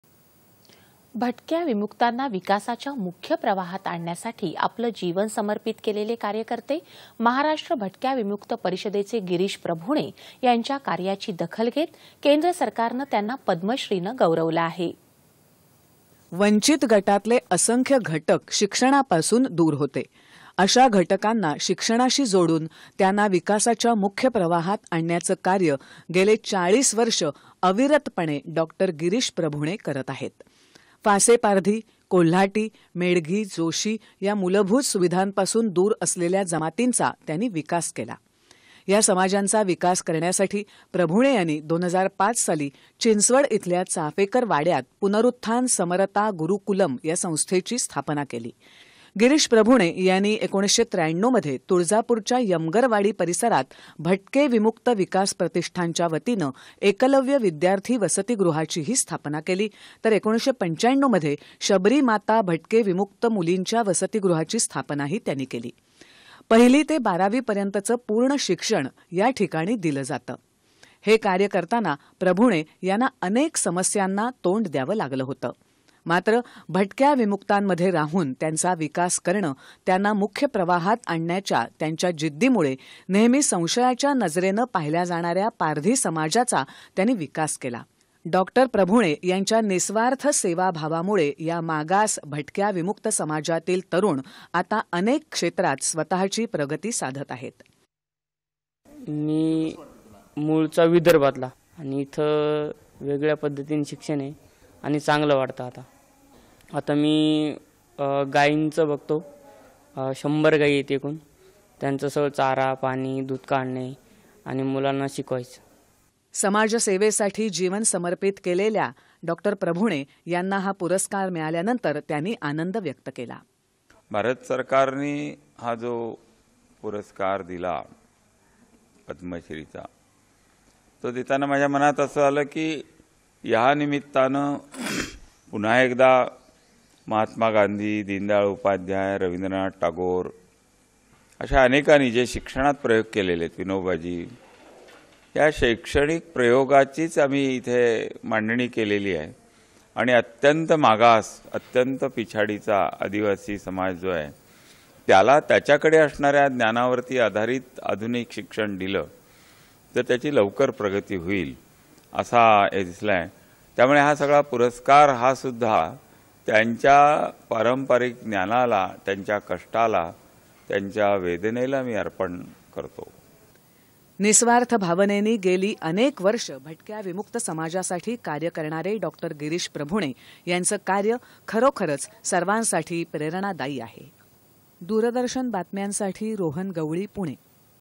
महाराष्ट्र भटक्या विमुक्त गिरीश प्रभुणे कार्याची दखल घेत, केंद्र सरकारनं त्यांना पद्मश्रीनं गौरवलं
भटक्या विमुक्तांना विकासाच्या मुख्य प्रवाहात आणण्यासाठी आपलं जीवन समर्पित केलेले कार्यकर्ते, महाराष्ट्र भटक्या विमुक्त परिषदेचे गिरीश प्रभुणे यांच्या कार्याची दखल घेत, केंद्र सरकारनं त्यांना पद्मश्रीनं गौरवलं आहे.